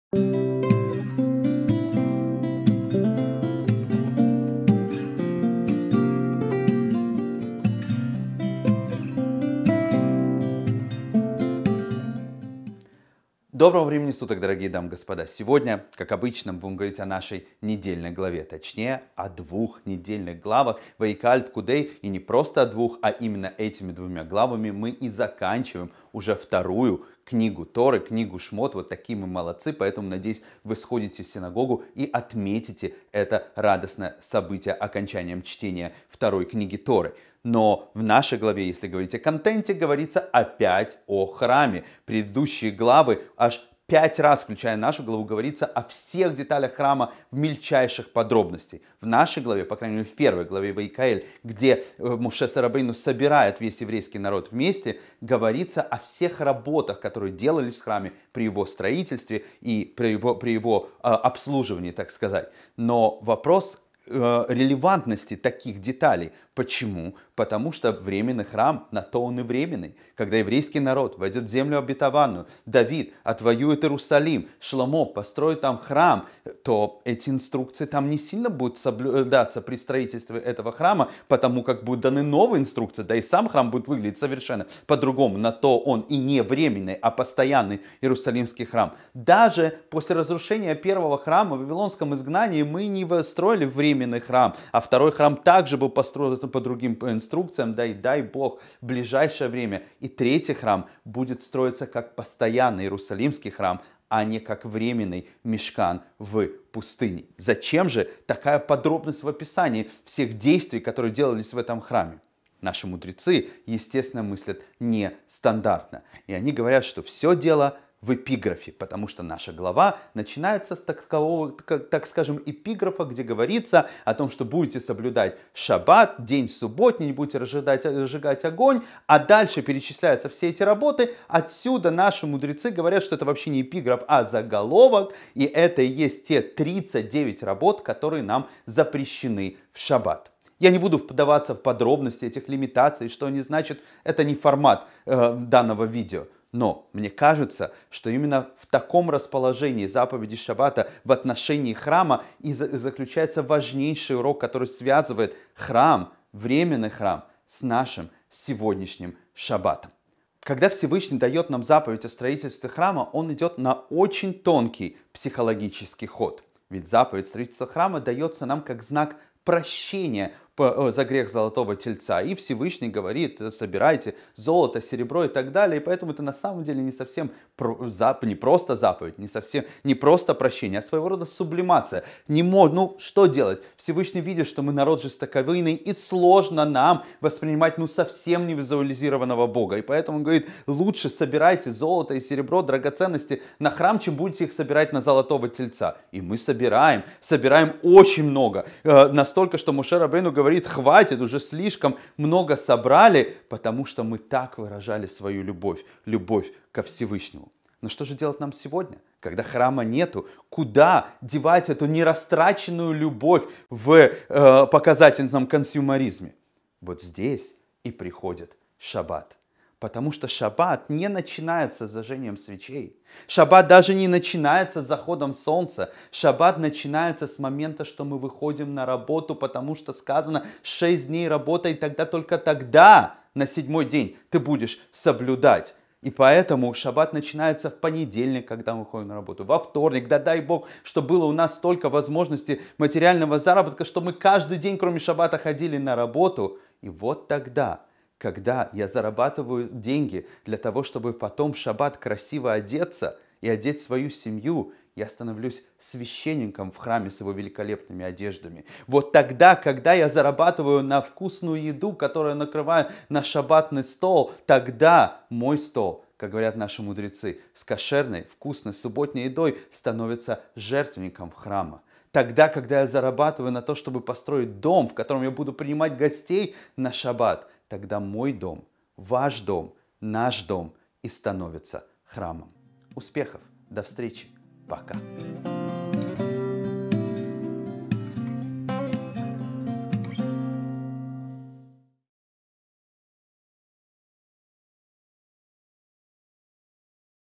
Мой Дом - Мой Храм. Ваякгель - Пкудей 5781 | Недельная глава с равом Альтшулем
Доброго времени суток, дорогие дамы и господа! Сегодня, как обычно, мы будем говорить о нашей недельной главе - точнее, о двух недельных главах, Ваякгель и Пкудей. Этими двумя главами мы заканчиваем уже вторую книгу Торы - Шмот.

И если говорить о контенте, в нашей главе говорится опять о Храме. В предыдущих главах, включая нашу, аж пять раз говорится об устройстве Храма в мельчайших подробностях. В первой из двух наших глав - Ваякгель, где Моше рабейну собирает весь народ вместе, говорится обо всех работах, которые делались в Храме при его строительстве и обслуживании.